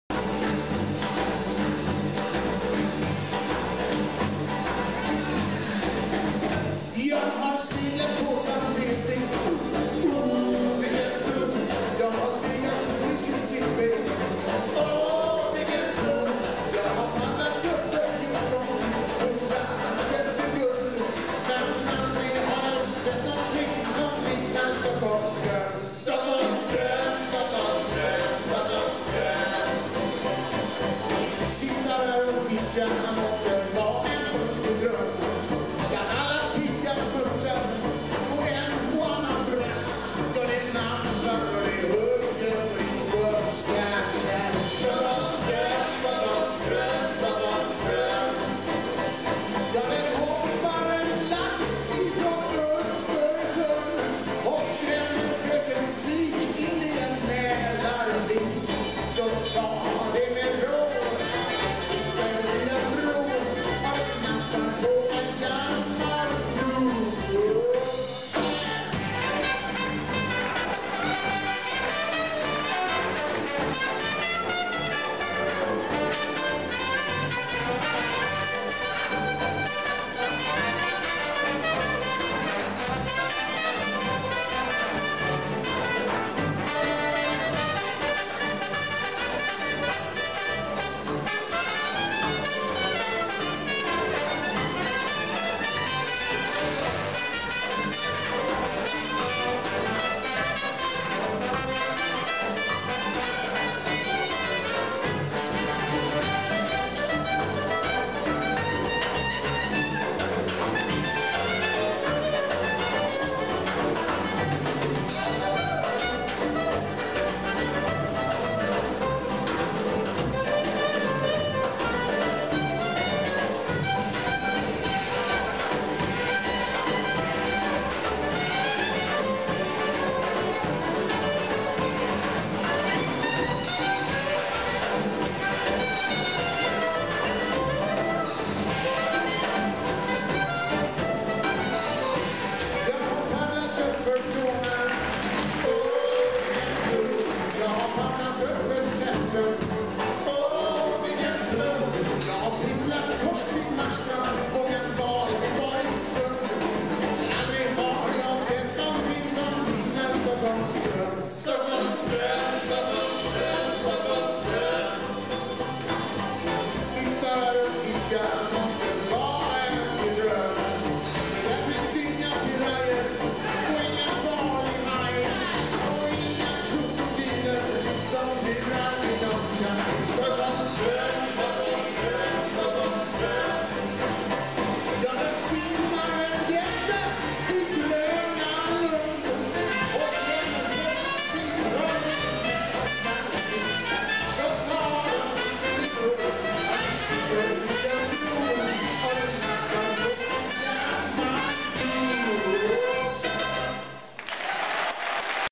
Stockholm Ström Second Line Jazzband
Second Line Jazzband  Göteborgs Jazzfestival
Niclas Carlson Jesper Albrektsson  0lof Skoog  Peter Bach  Anders Östlund  Anders Wasèn